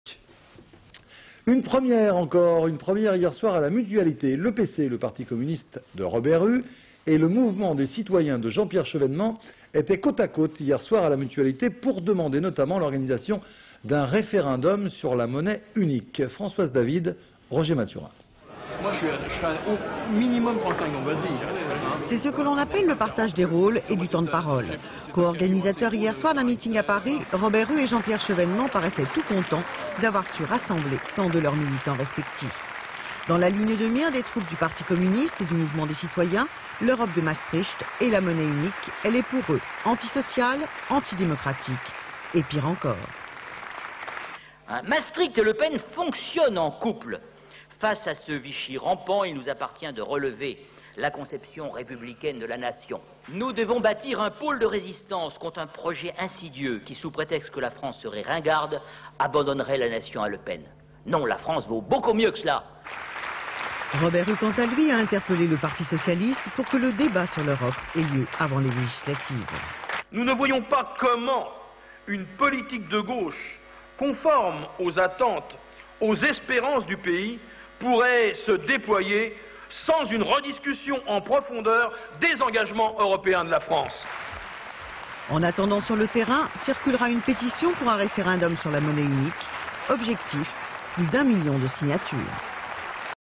Meeting PC/Mouvement des Citoyens
20 fevrier 1997
Robert HUE, secrétaire national du PCF, et Jean-Pierre CHEVENEMENT, président du Mouvement Des Citoyens, tenaient hier à La Mutualité à Paris un meeting pour demander notamment l'organisation d'un referendum sur l'instauration, dans le cadre du traité de Maastricht, d'une monnaie unique à laquelle ils s'opposent. politique; archive television; archive tv; ina; inna; Institut National de l'Audiovisuel; french tv Images d'archive INA